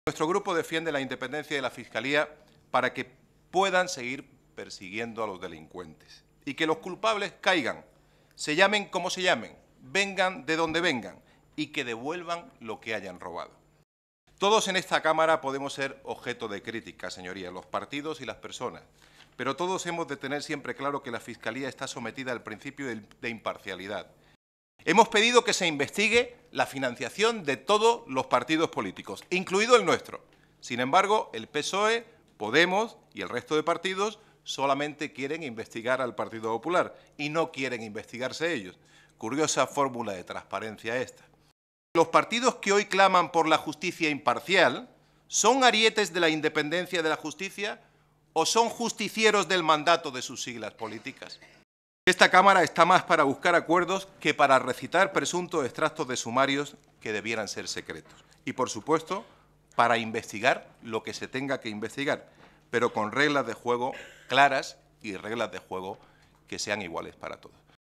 Defendemos la independencia de la Fiscalía para que puedan seguir persiguiendo a los delincuentes
Intervención de nuestro portavoz adjunto, Carlos Rojas, en la Comisión de Justicia del Congreso de los Diputados